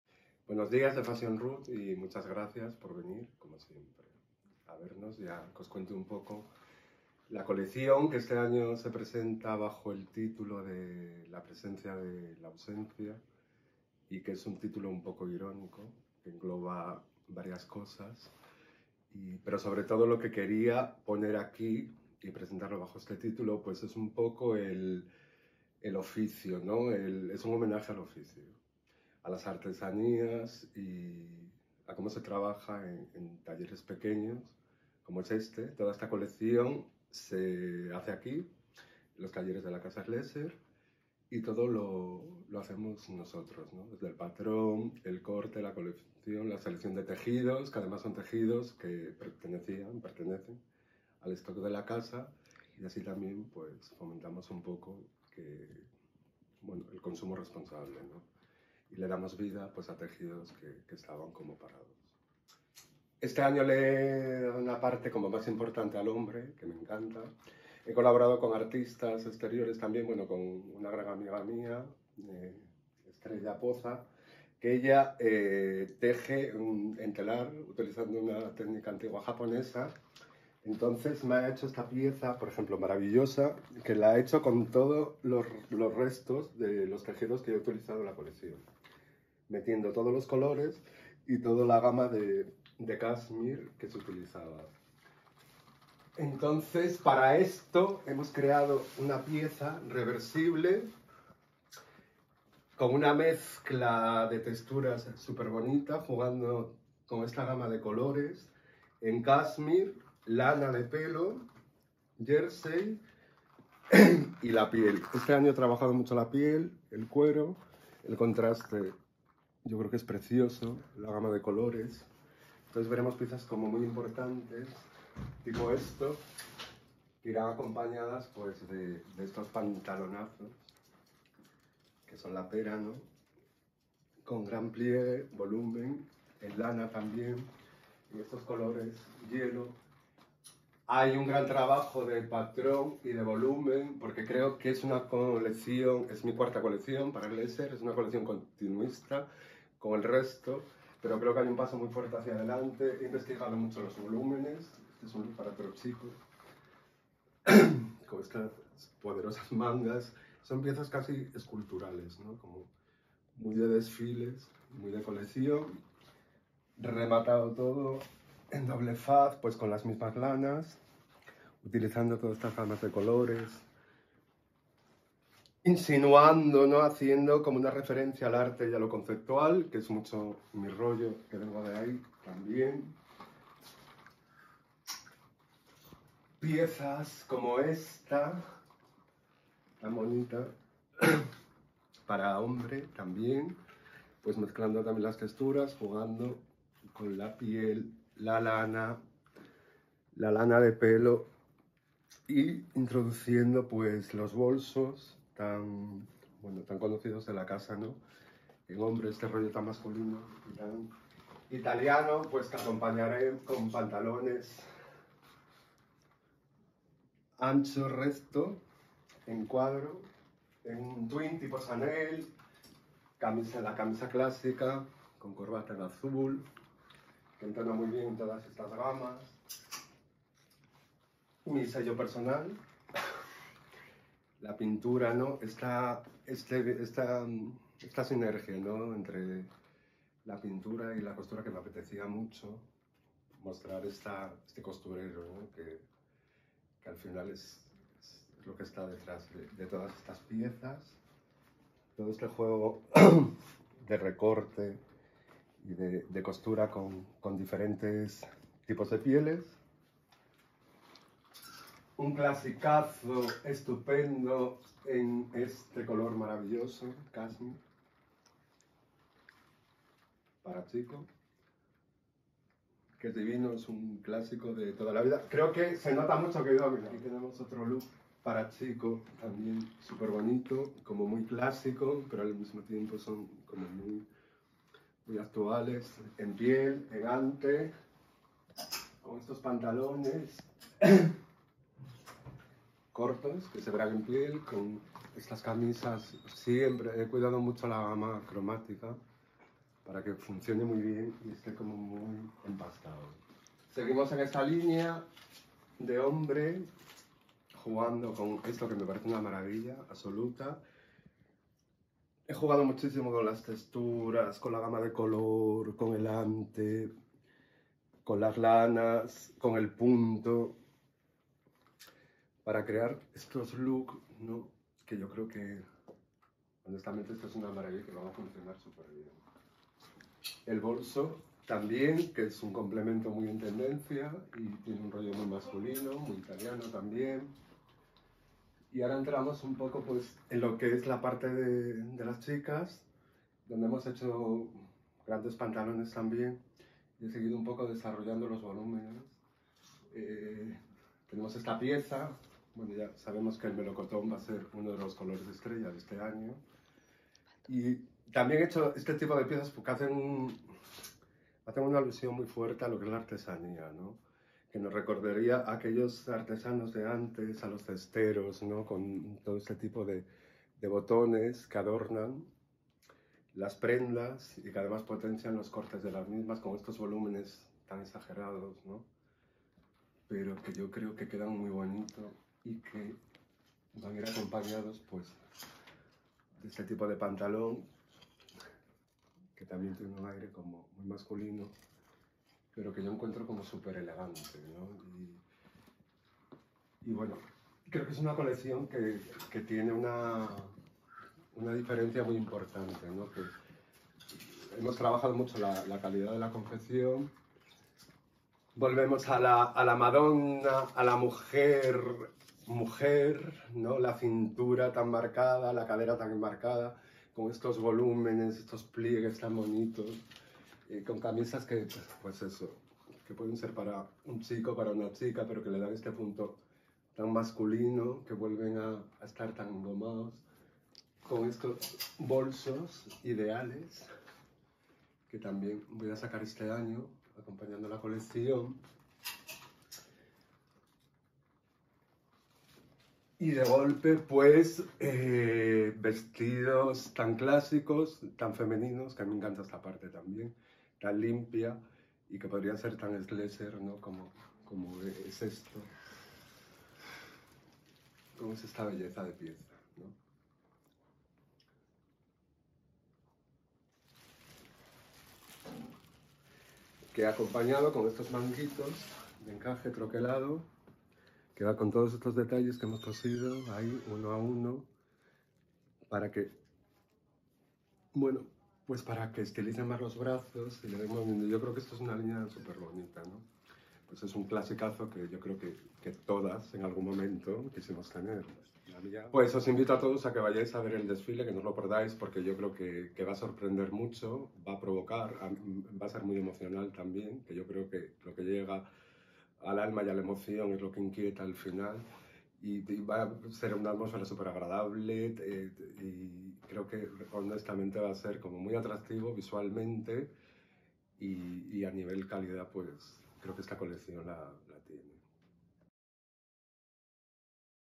Angel schlesser
Entrevista a Alfonso Pérez Director creativo de ANGEL SCHLESSER , en su atelier de MADRID, días antes de presentar la colección "Presencia de la Ausencia"  FW 2023/2024. Desdile en la ciudad dentro del calendario oficial Mercedes-Benz Fashion Week Madrid .